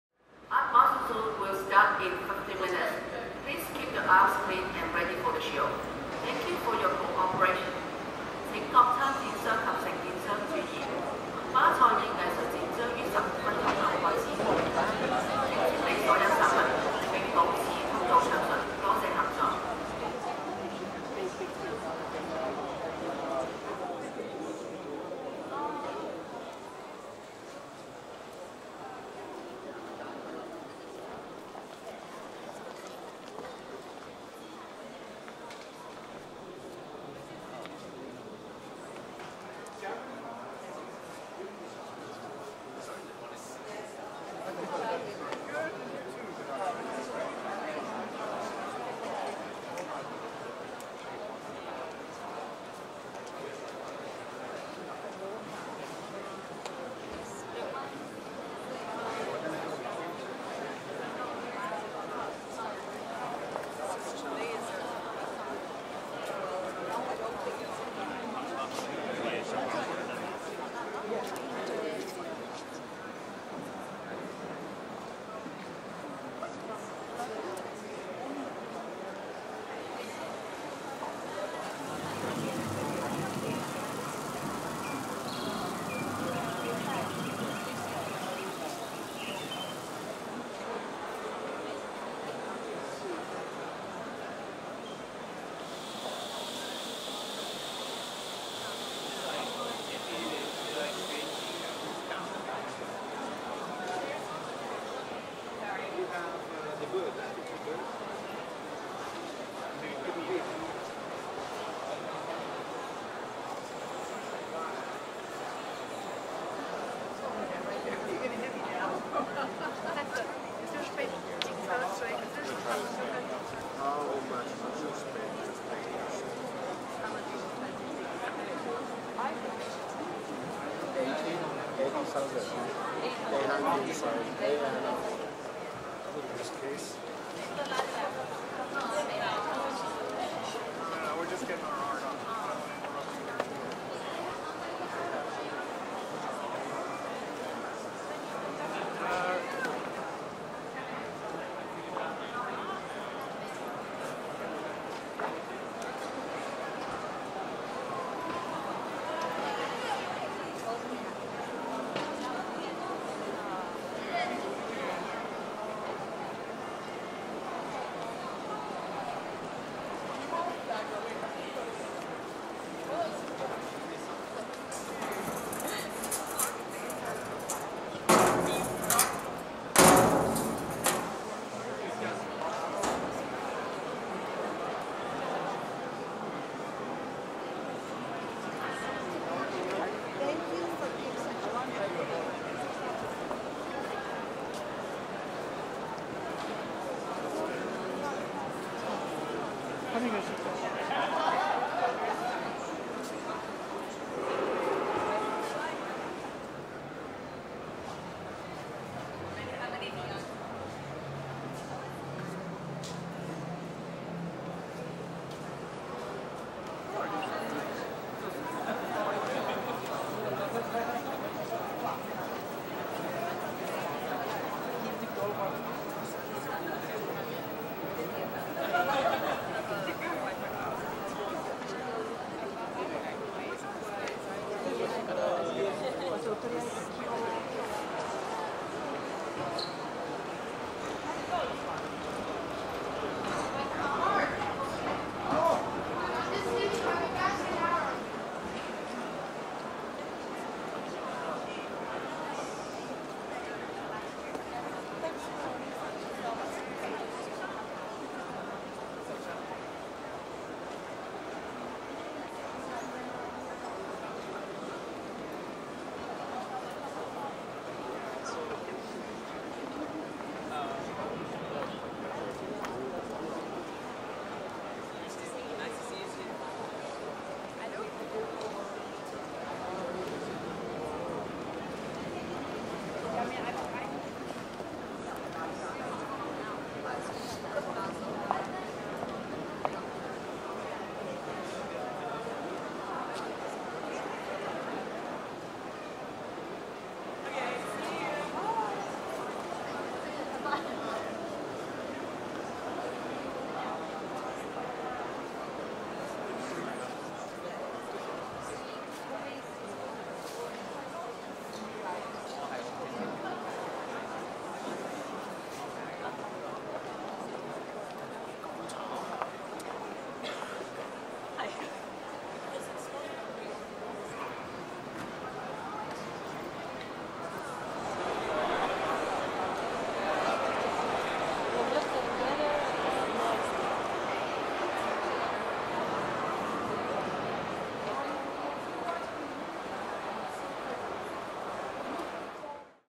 Art Basel Hong Kong 2016
In this video, we attend the Private View of Art Basel in Hong Kong 2016. It's Art Basel's fourth edition in Hong Kong. The art fair brings together 239 exhibitors, half of them from Asia and Asia-Pacific, half of them from the rest of the world. The Galleries sector features artworks ranging from Modern to contemporary art.

Art Basel Hong Kong 2016, Private View, March 22, 2016.